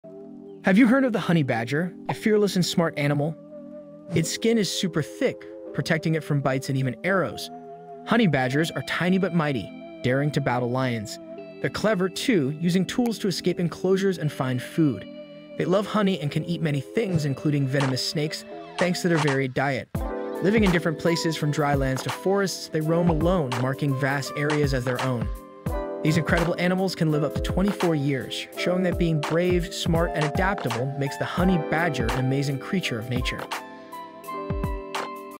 Unyielding Spirit: The Honey Badger's Tale
Dive into the world of the honey badger, nature's fearless warrior, in "Unyielding Spirit: The Honey Badger's Tale." This documentary-style video unravels the mysteries of one of the most tenacious animals on the planet. Known for its incredible strength, intelligence, and resilience, the honey badger has captured the admiration and curiosity of many. From its unique hunting strategies to its remarkable ability to withstand venom from snakes and bees, this video showcases the honey badger in its natural habitat, revealing why it's considered the most fearless animal in the world. Join us as we explore the life, challenges, and survival tactics of the honey badger in the wilds of Africa and beyond.